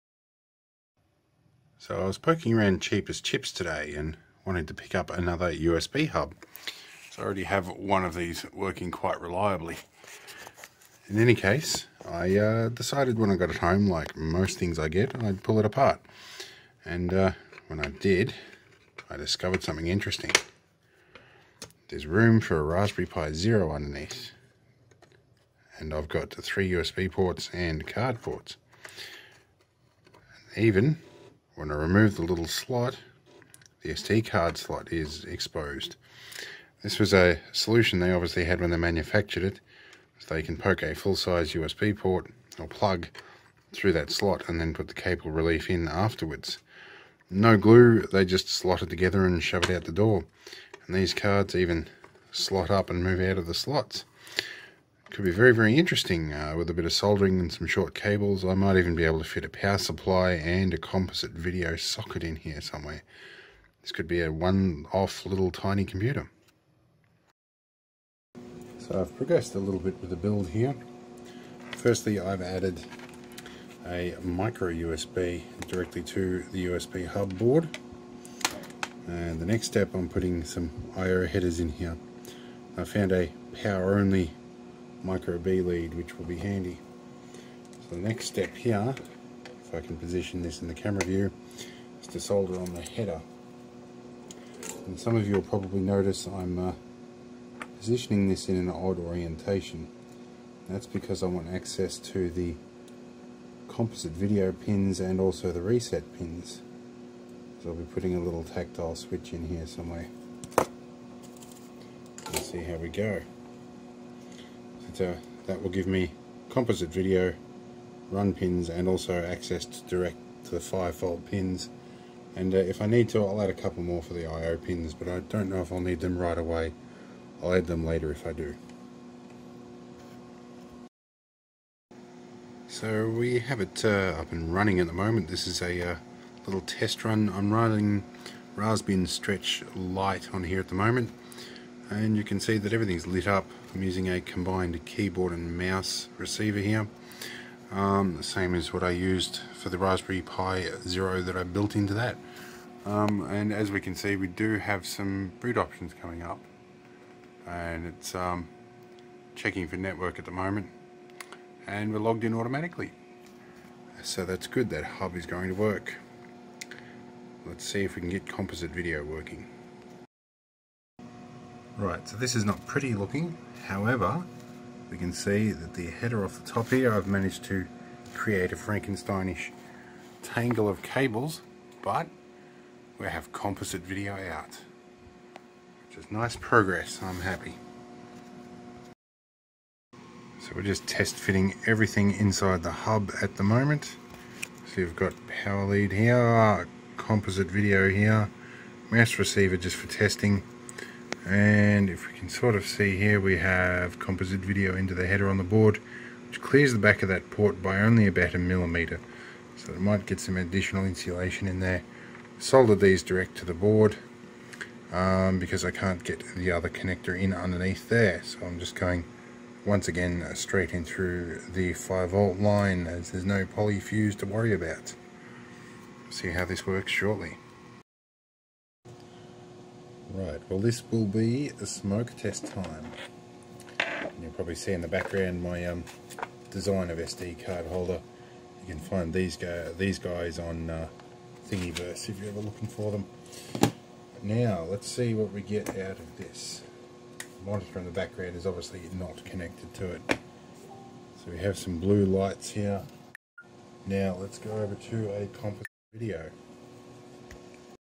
Raspberry Pi Zero W hidden in a cheap USB hub
I discovered that the USB hub from cheap as chips, had plenty of spare space inside. So I decided to cram a Pi Zero W in there and make it into a portable card/usb stick copier.